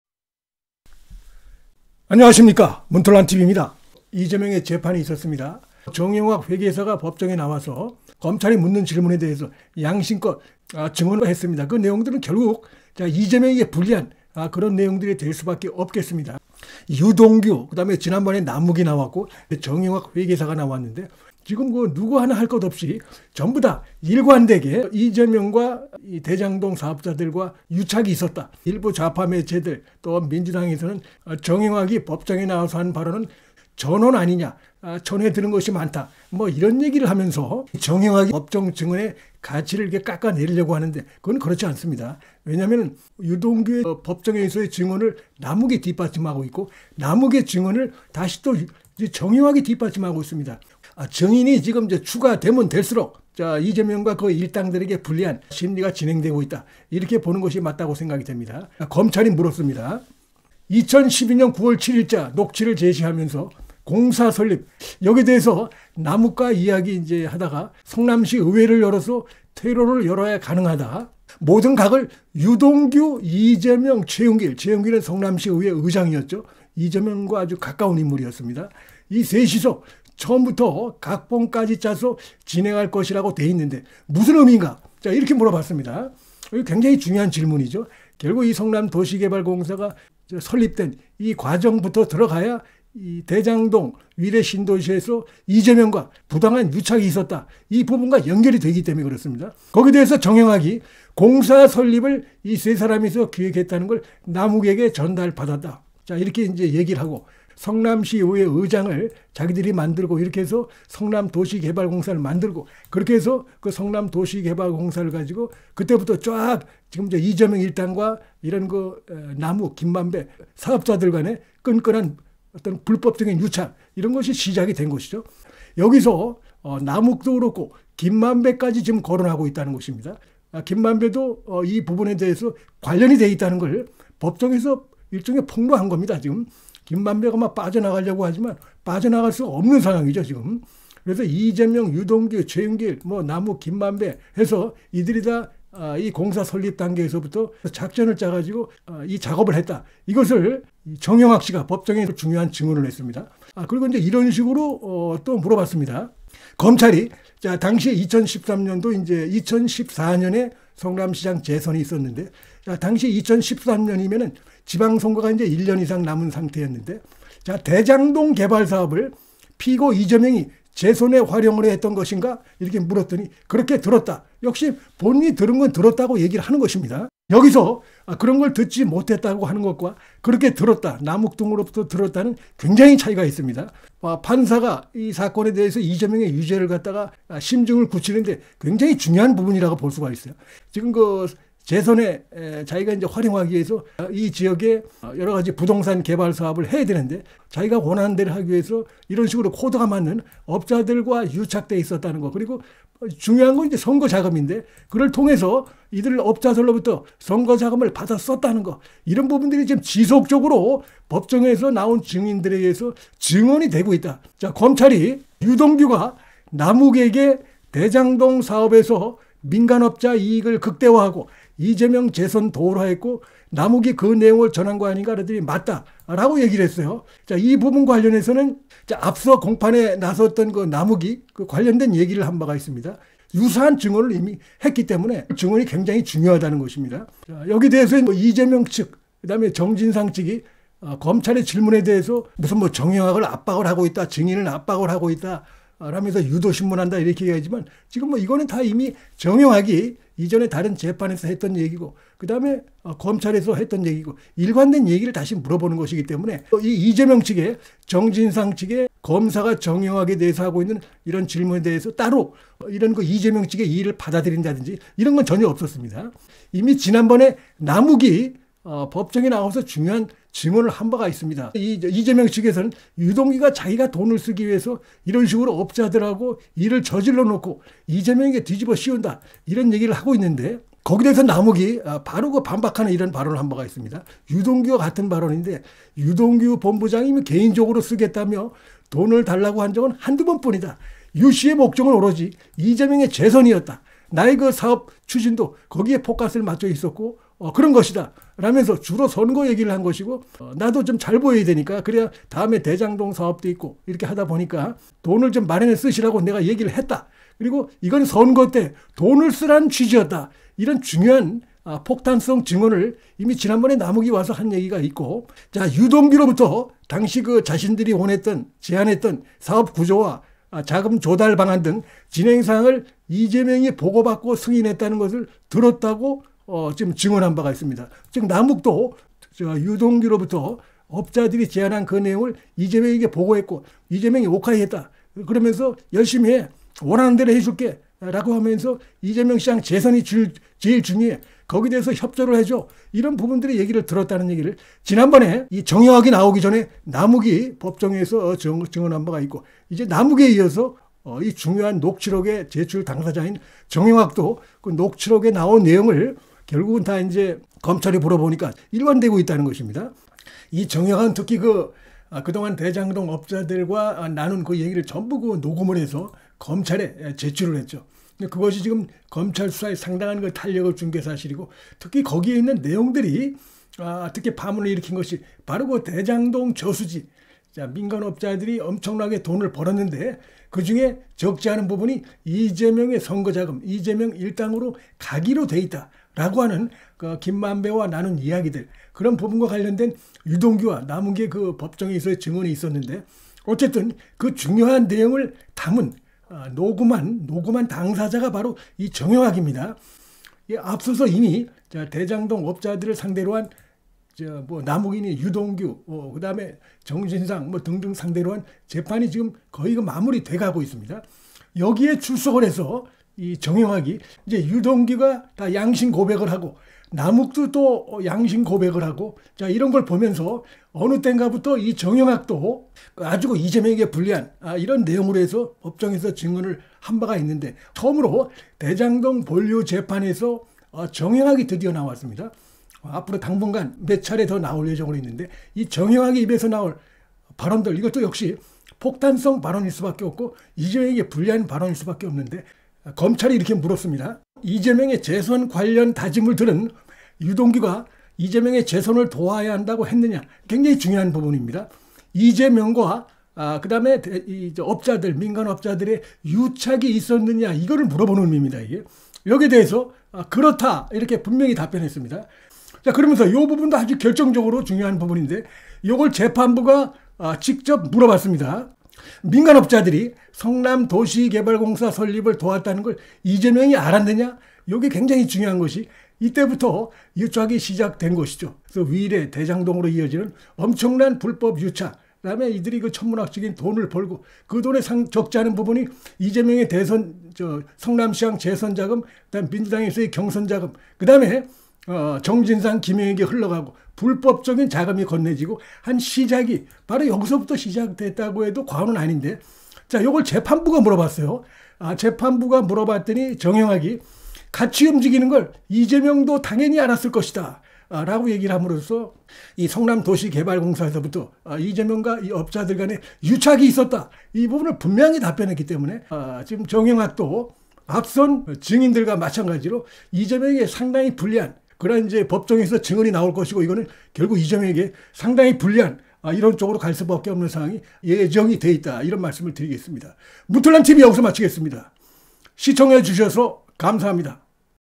정영학, 이재명 비자금 저수지 녹취 파일 공개한다!!
정영학,  유동규 남욱 증언 뒷받침!!